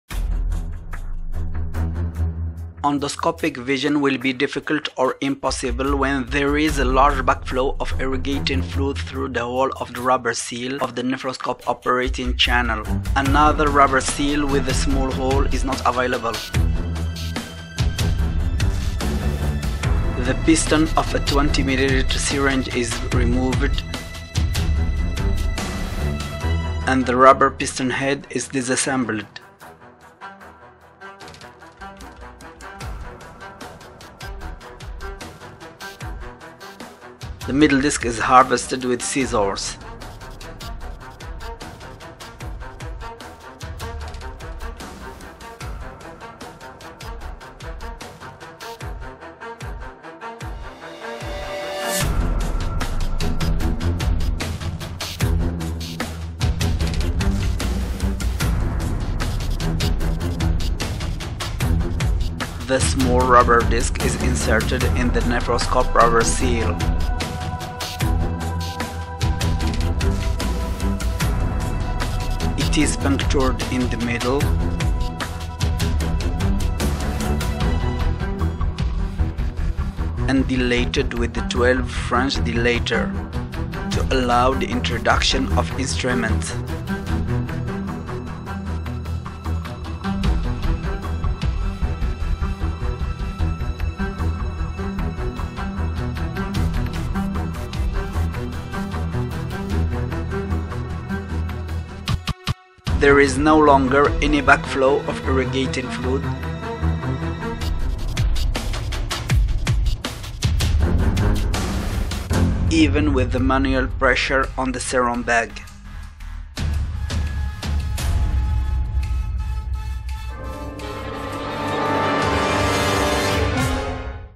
A trick for a watertight nephroscope rubber seal
Endoscopic visibility will be difficult or impossible when there is a large back-flow of irrigating fluid, through the hole of the rubber seal of the operating channel of the nephroscope.
another rubber seal with a small hole is not available. The piston of a 20 ml syringe is removed and the rubber piston head is disassembled. the middle disk of this rubber head is harvested with scissors. the small rubber disk is inserted in the nephroscope's rubber seal. there is almost no leak through the rubber seal's hole.